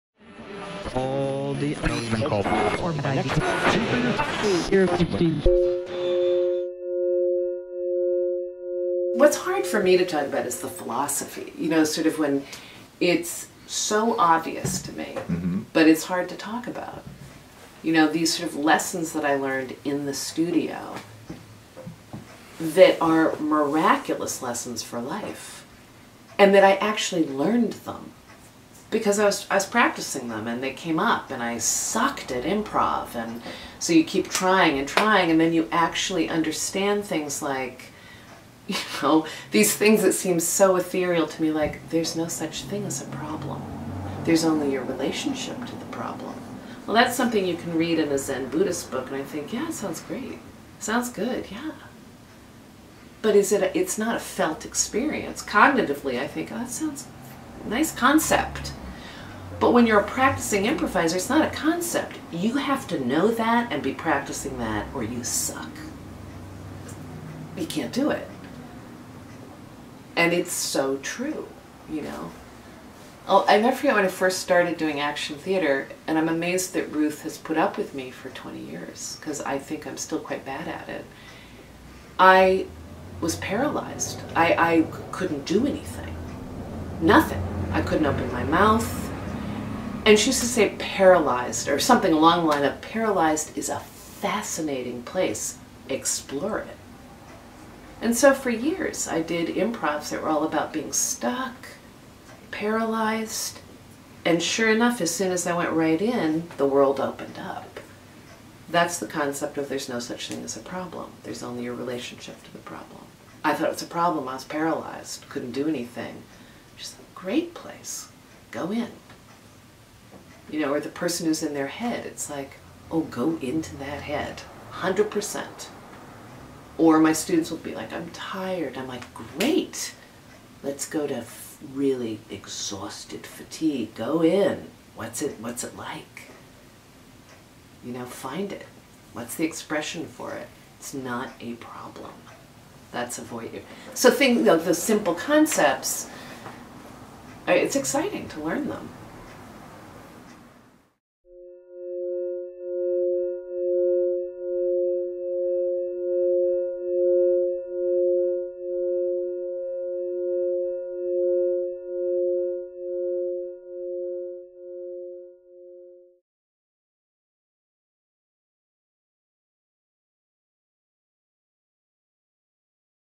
Episode 30: Simple Concepts
Actor, Director and Movement Specialist Sarah Hickler talks about learning through improvisation.